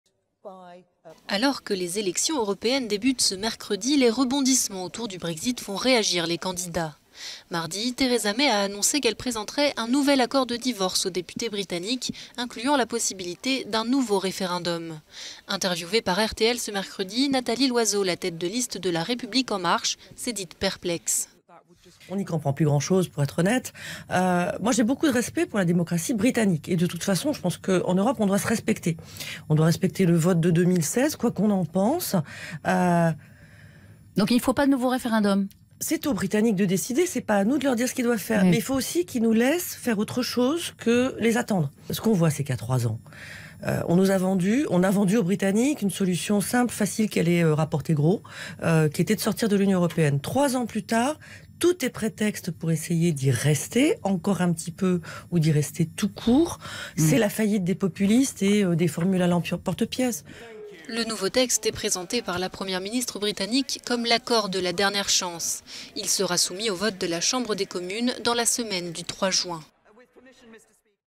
Nathalie Loiseau sur le Brexit : "c'est la faillite des populismes"
La tête de liste du parti La République En Marche s'est dite perplexe, après la présentation du nouvel accord de Brexit par Theresa May.…